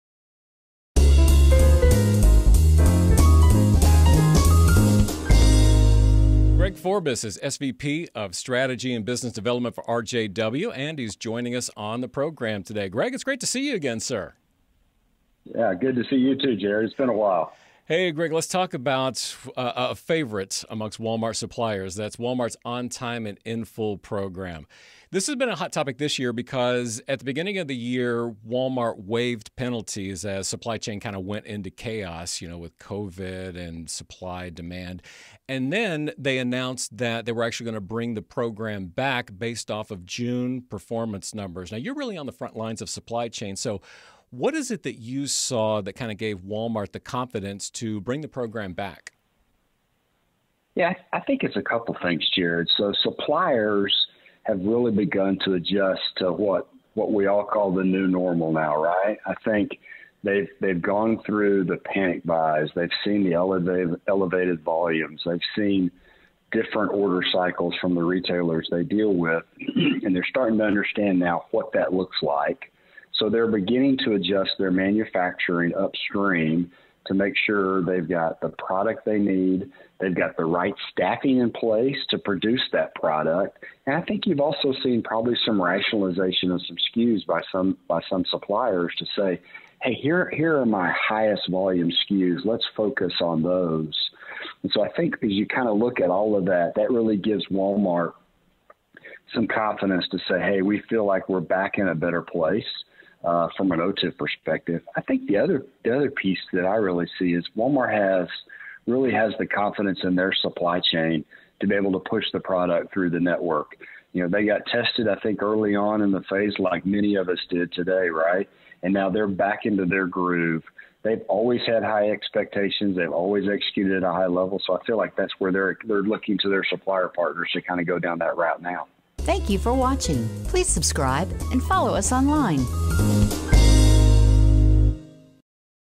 RJW discusses Walmart Bringing OTIF Back
Greg Fobis of RJW Logistics Group shares his thoughts on why Walmart brought the OTIF program back based on June performance numbers.